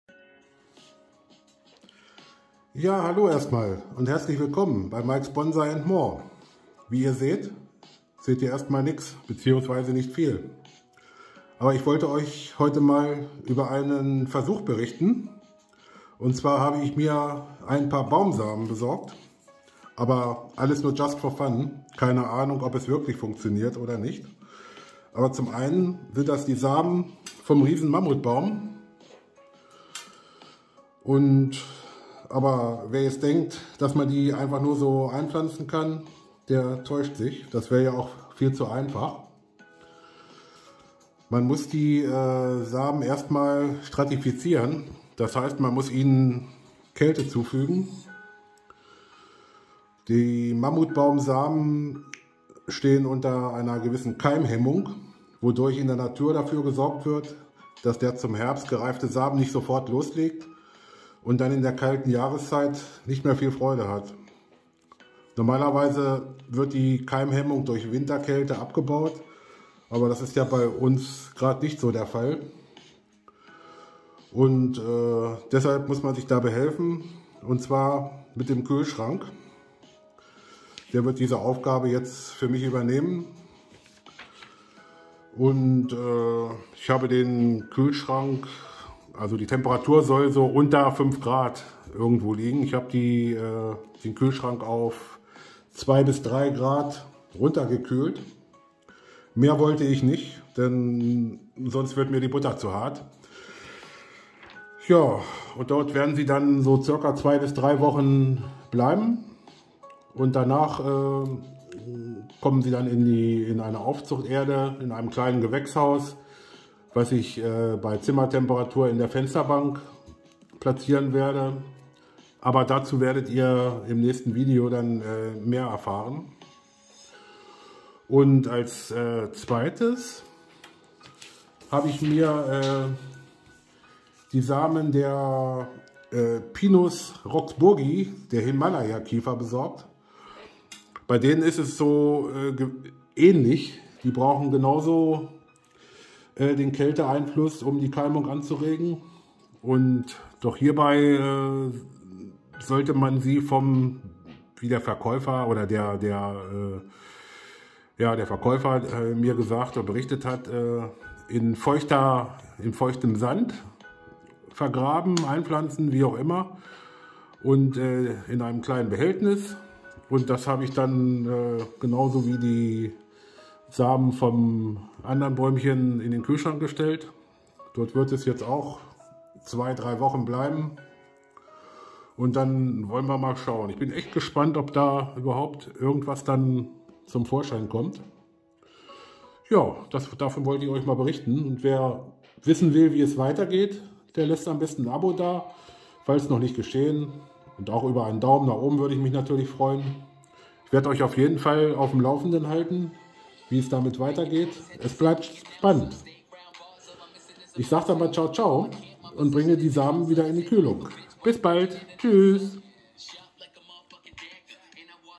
Riesenmammutbaum/Himalaya Kiefer Samen Teil1
Ich habe mir ein paar Baumsamen vom Mammutbaum und der Himalaya Kiefer besorgt und versuche sie zum Keimen zu bekommen